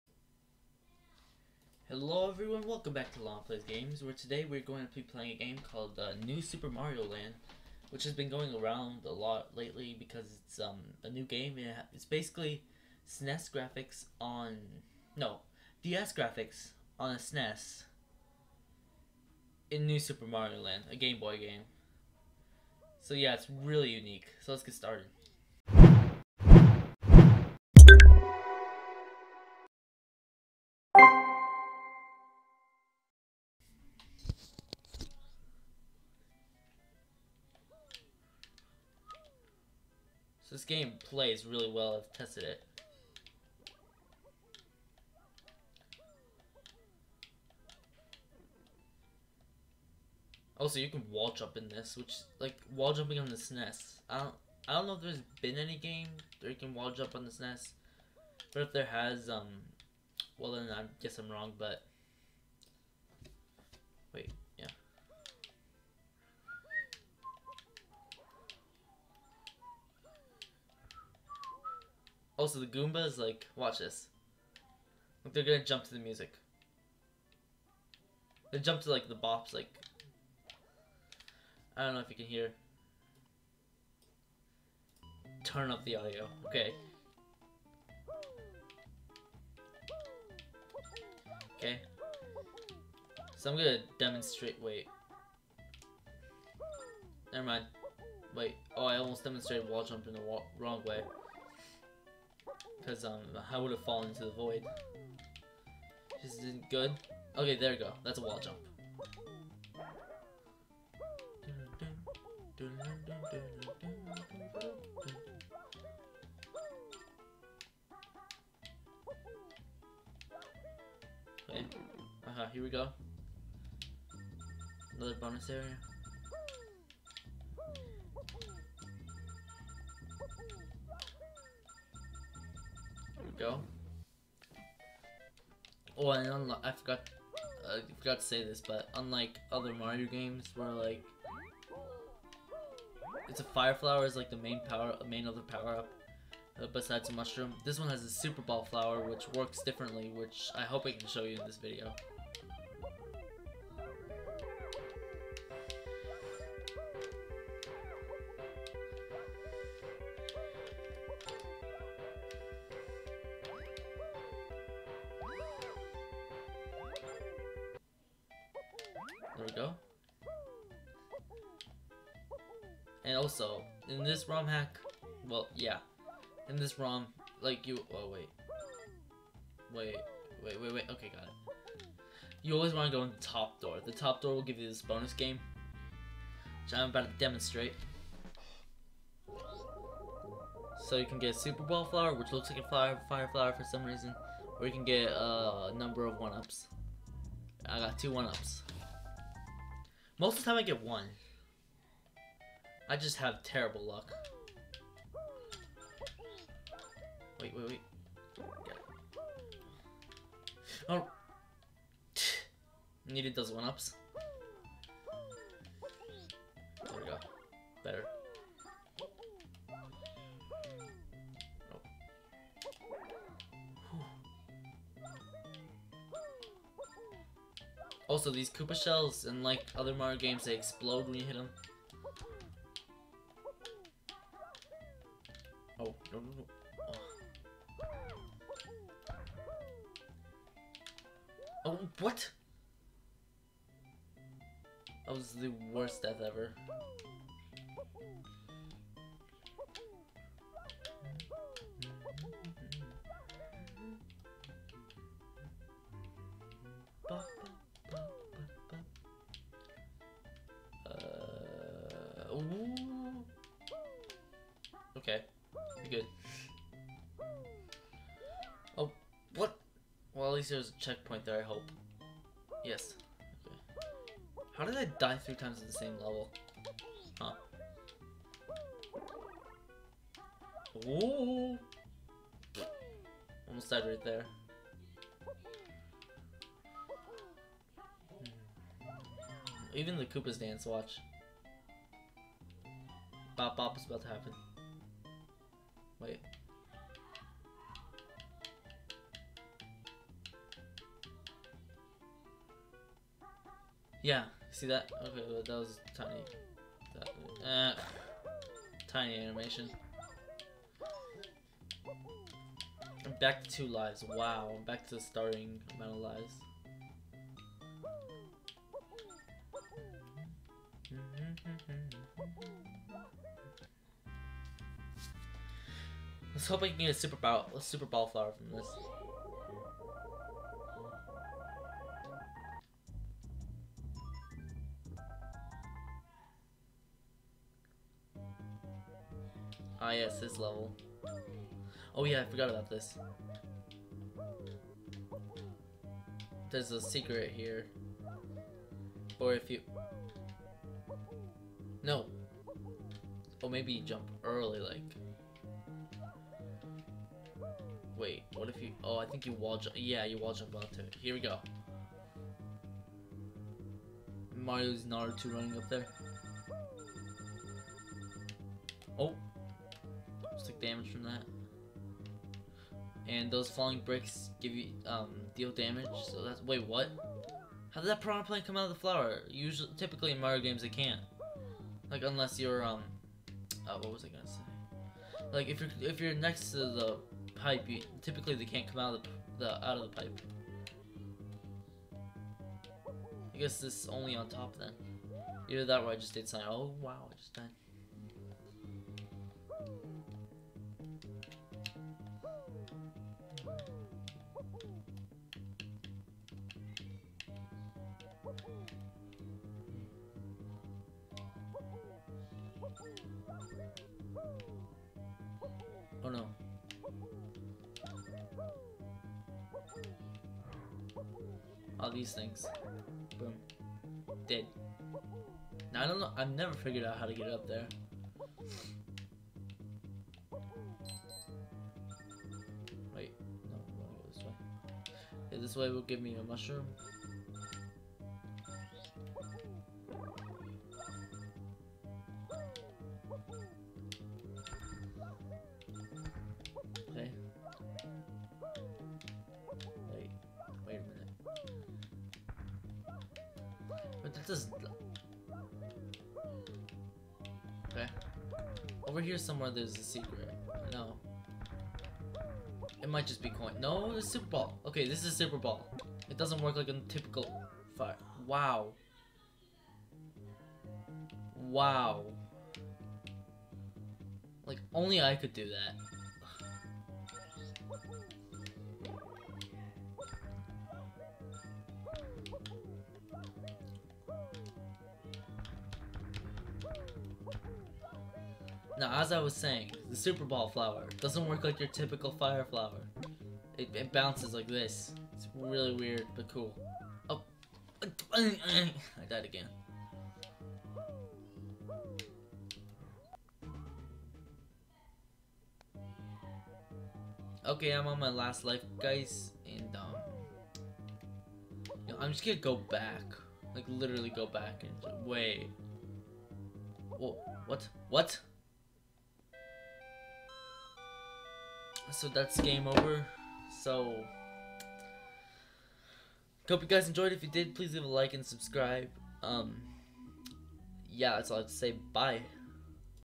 This game is EPIC
It's a gameboy game on a snes with ds graphics. It's awesome.